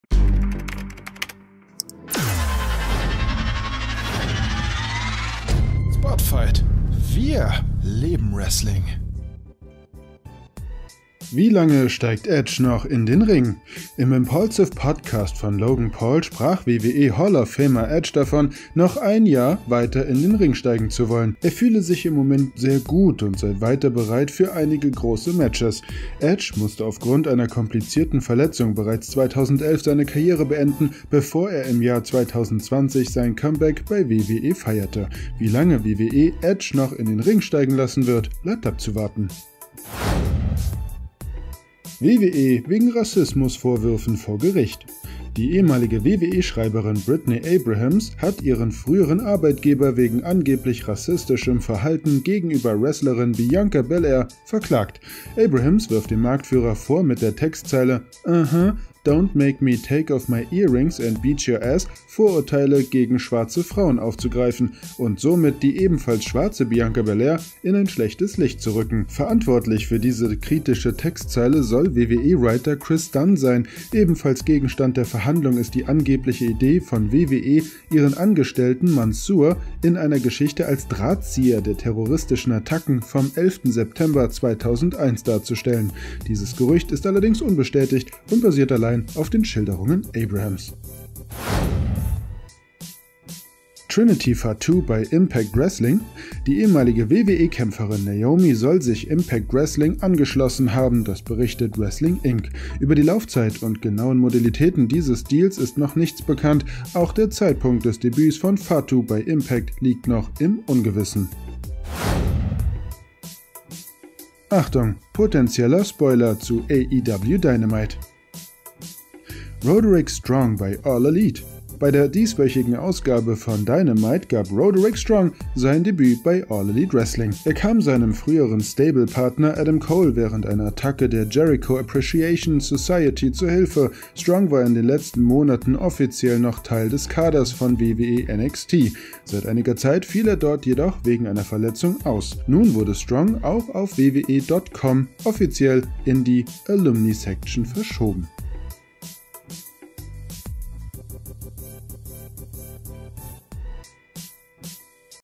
WWE wegen Rassismus-Vorwürfen vor Gericht! Edge über Karriere-Ende? (WWE News, Wrestling News)
sub_confirmation=1
Unsere Redakteure: Jonas Dietz, Florian Frense, Gill Mertens, Paul Antwerpes

𝗤𝗨𝗘𝗟𝗟𝗘𝗡:
Quellen: Impaulsive Podcast, Sportskeeda, Wrestling INC., AEW Dynamite, WWE.com